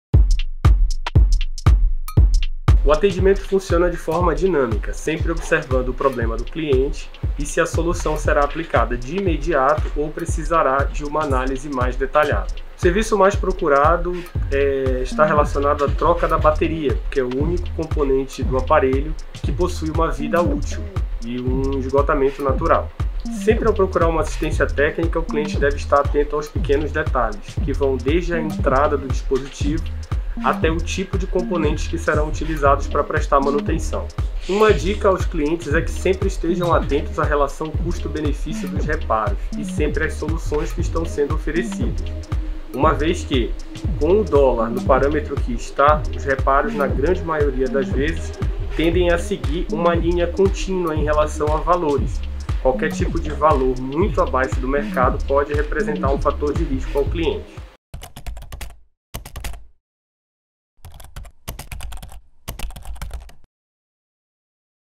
Empresário Lennon Muniz fala sobre os desafios da assistência técnica de celulares
Empreendedor explica as exigências do mercado em que empresas trabalham com dispositivos contendo informações pessoais